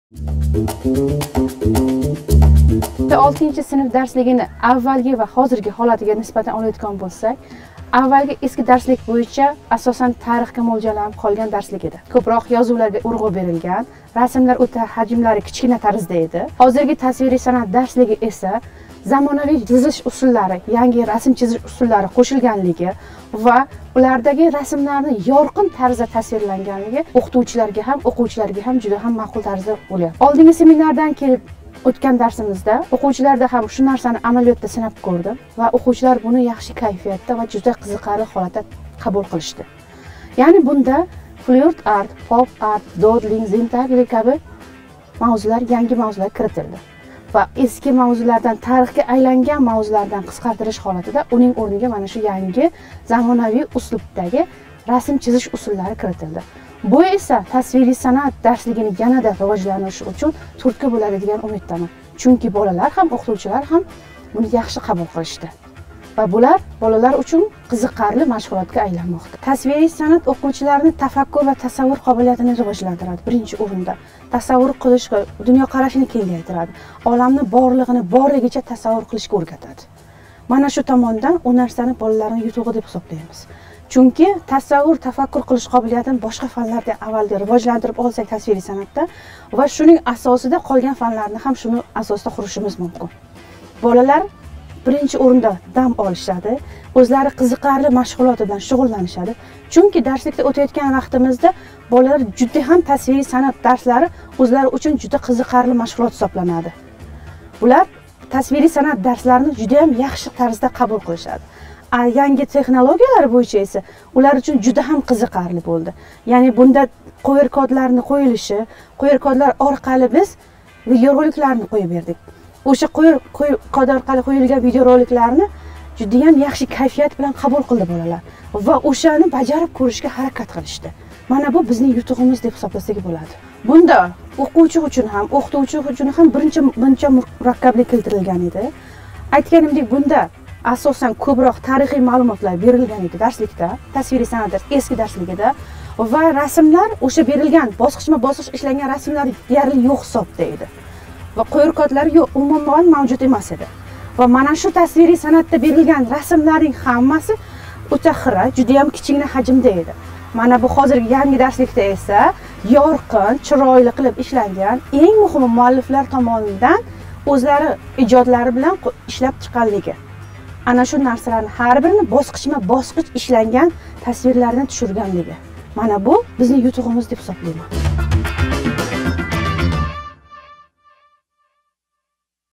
Maftuna Mardonova , Samarqand viloyati Toyloq tumani 3-maktabning Tasviriy san'at fani o'qituvchisi
Darslik oʻqituvchi va oʻquvchini ijod qilishga undaydi
STREET ART, POP ART, 3D texnikasi, GRAFITTO, ZENTAGLE VA DOODLING, Kompyuter grafikasi, haykaltaroshlik va yangi fotografiya kompozitsiyasi darslikni yana ham qiziqarli va sermazmun boʻlishiga asos boʻlgan. Darslik oʻqituvchi va oʻquvchini ijod qilishga, kreativ yondashishiga va oʻz ustida koʻproq ishlashga undaydi.